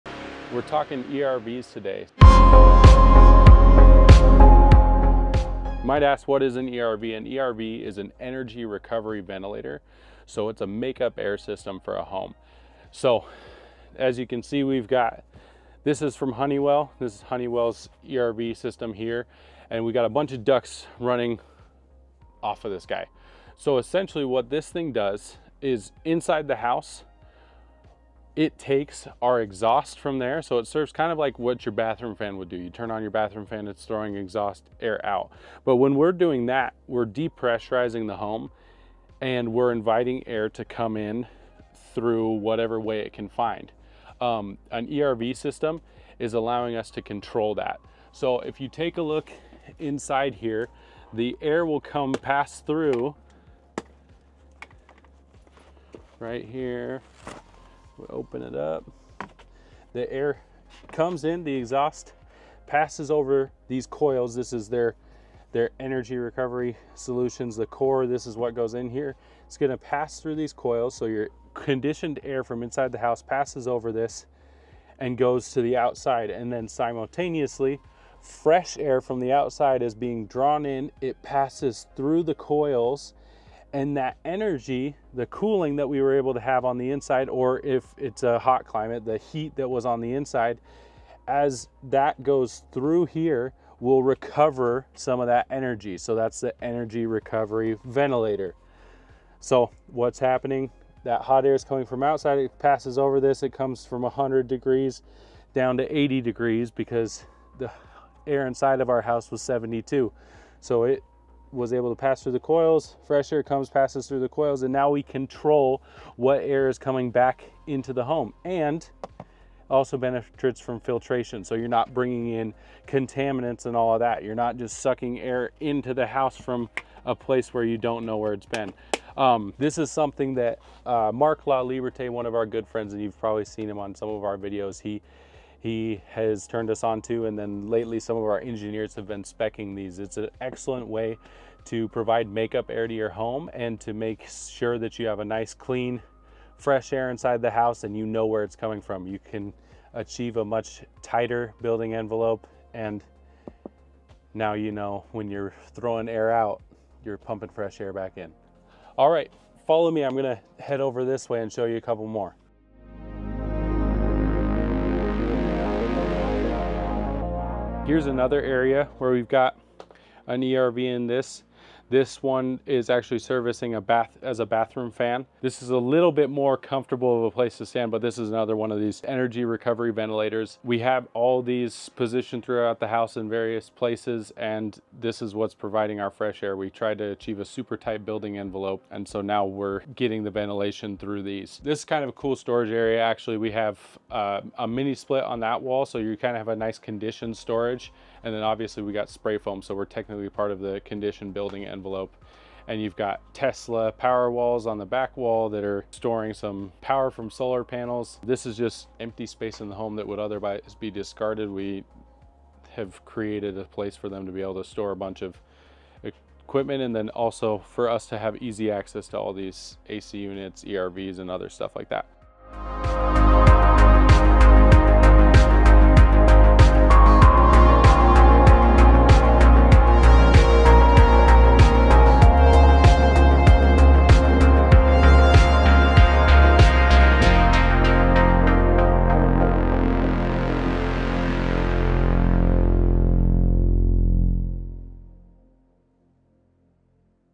What is an ERV System? | Energy Recovery Ventilator | Citrus Point | AFT Construction
The ERV system (Energy Recovery Ventilator) can be critical for improving indoor air quality and energy efficiency at Citrus Point. An ERV system helps manage ventilation by exchanging stale indoor air with fresh outdoor air while recovering energy. This ensures your home stays comfortable, energy-efficient, and properly ventilated, especially in today’s well-insulated, airtight buildings.

Balancing humidity, reducing energy consumption, and maintaining indoor air comfort, unlike traditional ventilation systems, an Energy Recovery Ventilator transfers heat and moisture between incoming and outgoing airstreams, allowing for energy-efficient ventilation.